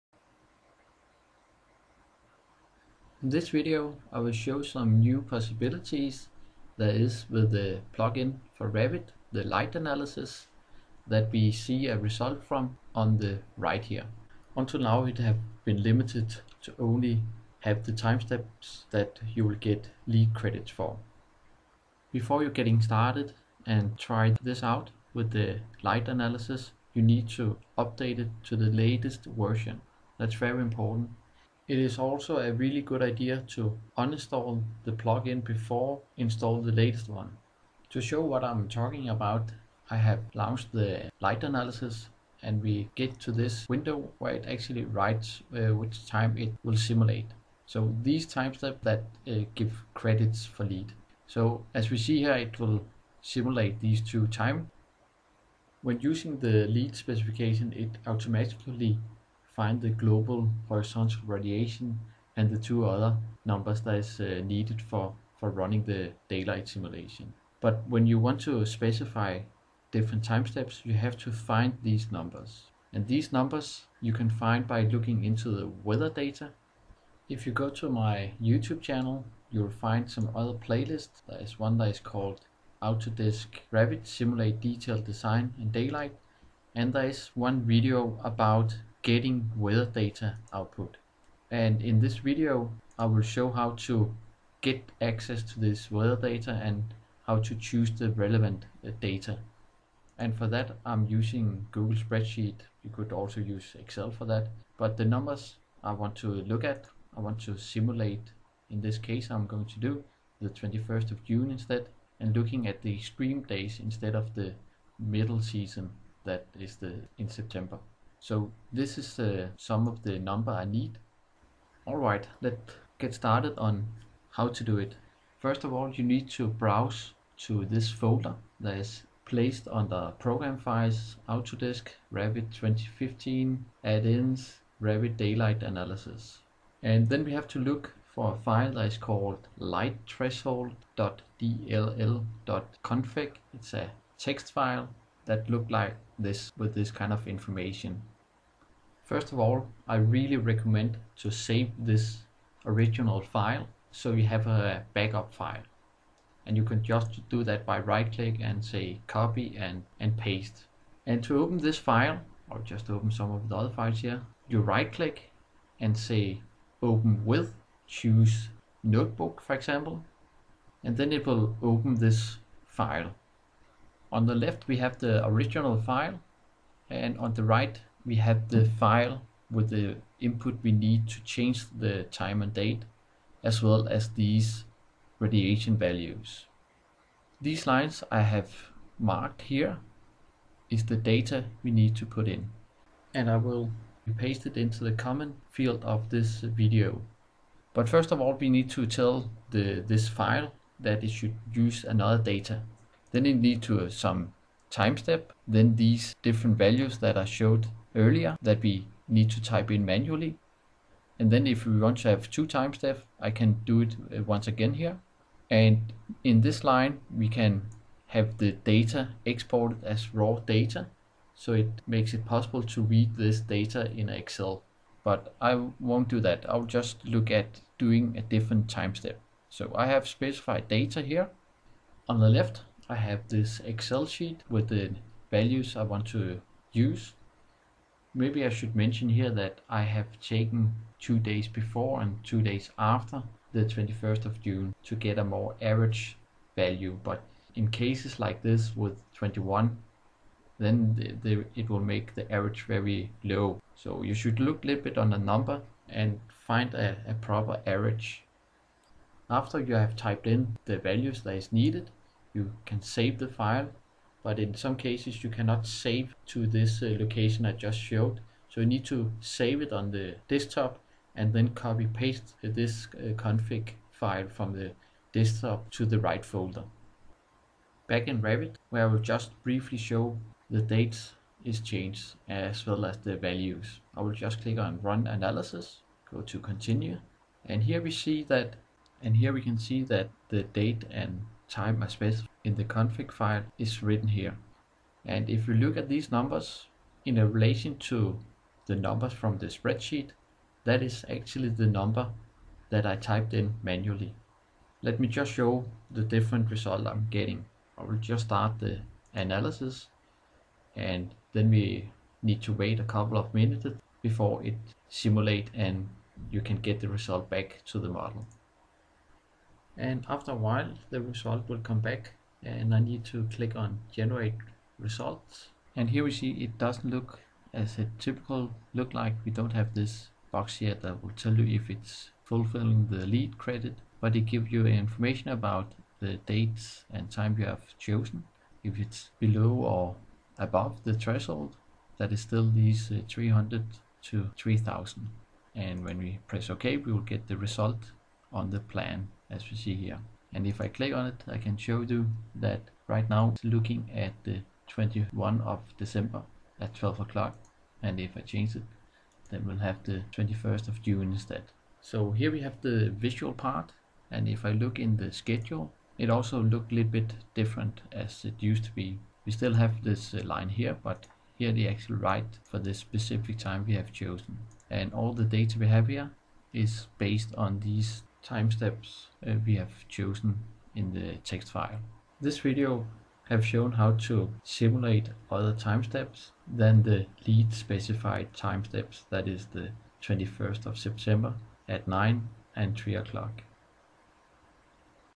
Autodesk Revit DA - Daylight simulation with custom Date+Time with Light Analysis (Illuminance/lux)
Link to the config file from the video (it was not possible to copy/paste the text here because of the brackets):